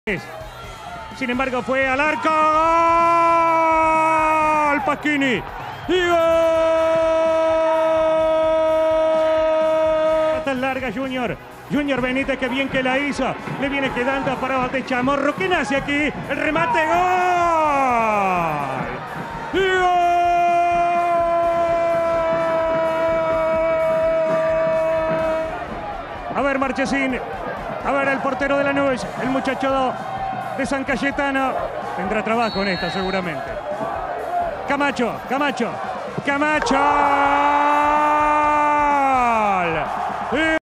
Defensa y Justicia 1 Lanus 2 Fecha 12 Torneo de Transición 2014
DEFENSA 1 (Camacho)
LANUS 2 (Pasquini - Valdez Chamorro)